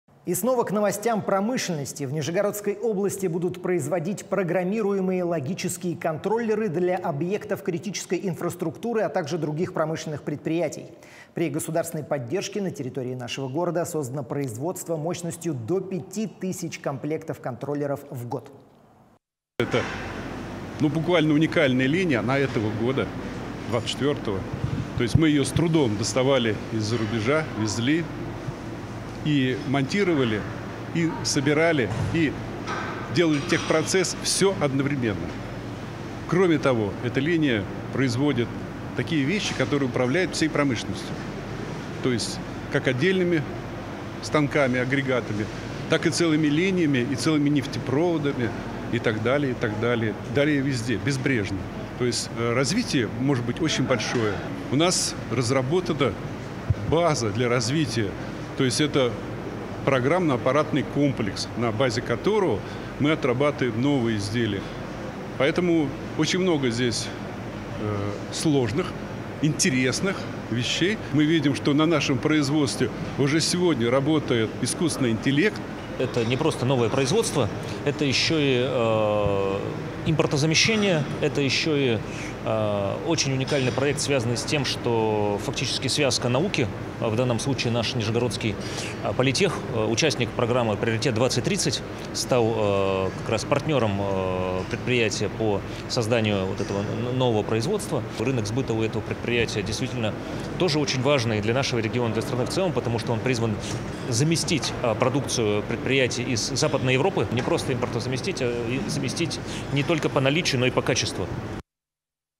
В Нижегородской области будут производить контроллеры для объектов критической инфраструктуры1
В Нижегородской области будут производить программируемые логические контроллеры для объектов критической инфраструктуры, а также других промышленных предприятий. При государственной поддержке на территории нашего города создано производство мощностью до пяти тысяч комплектов в год.